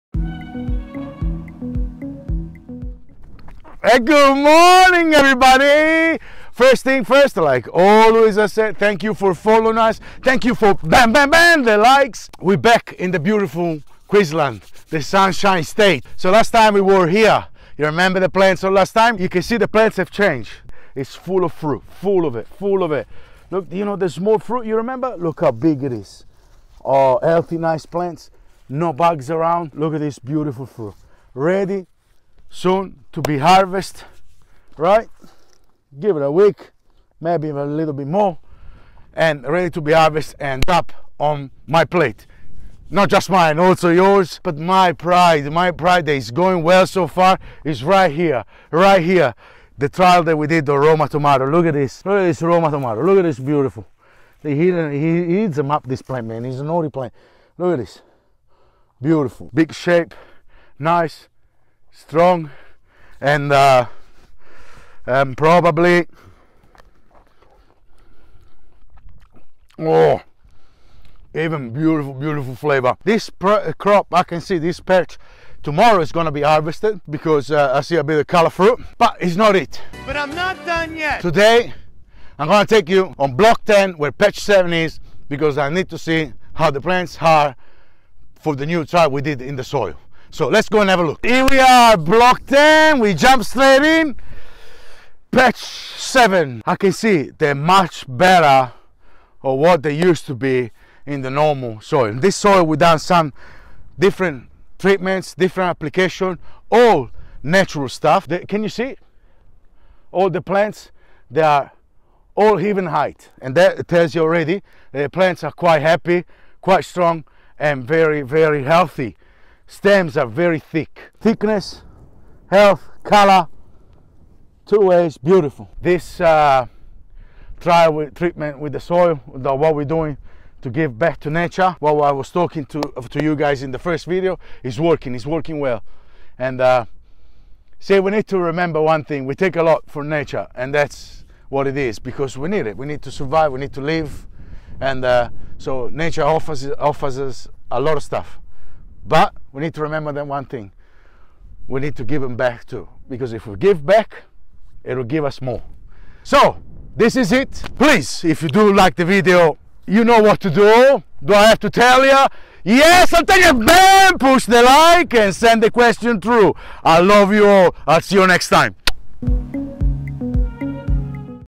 Antonio's Report - QLD Pride: Tomato Season Update
AntoniosReport! Antonio is back in BOWEN, QLD and reveals all about the current TOMATO SEASON! 👨‍🌾🍅